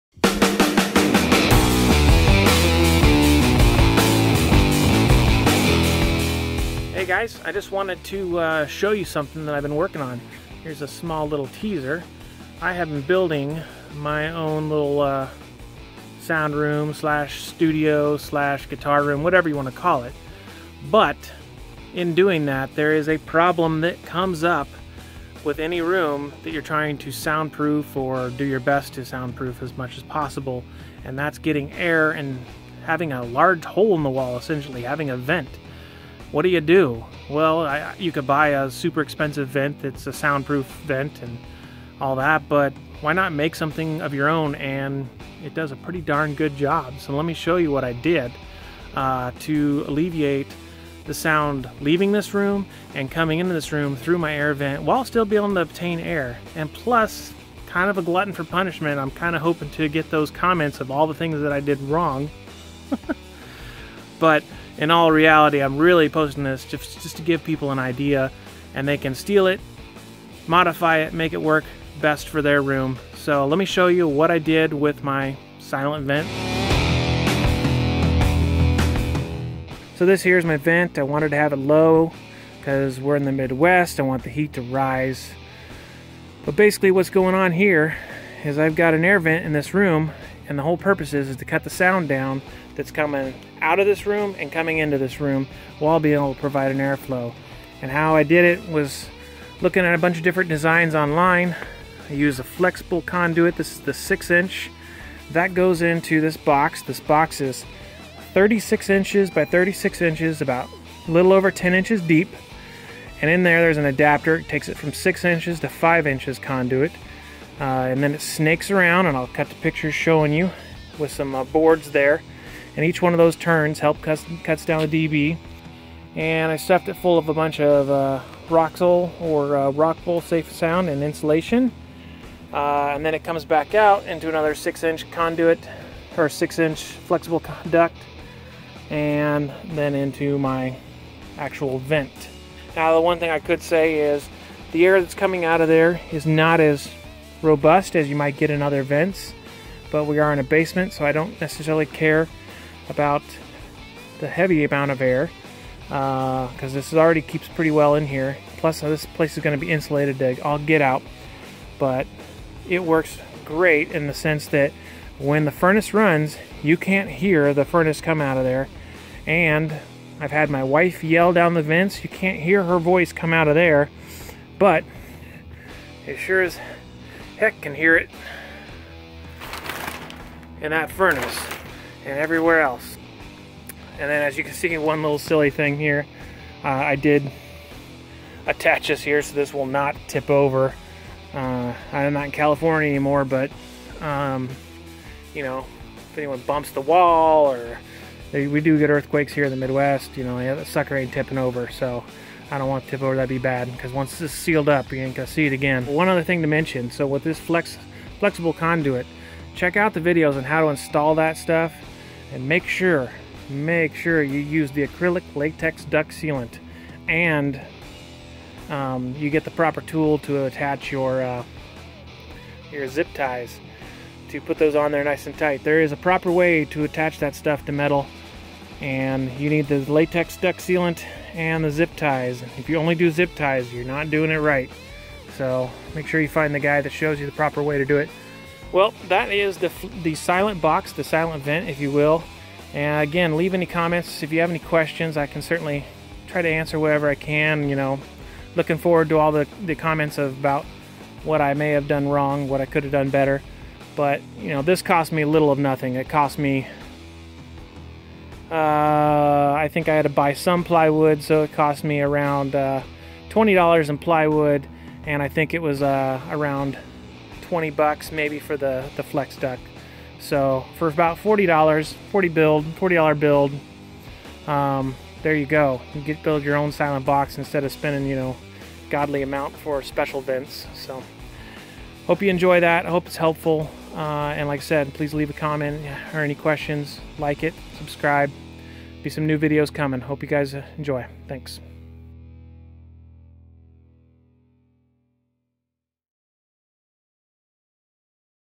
How to Build a Silent Air Vent For a Recording Studio or Home Theater
This video shows how to build a silent air vent for you future sound room or Home Theater.  This allows the air flow while keeping the sound in and sound out from other parts of your house.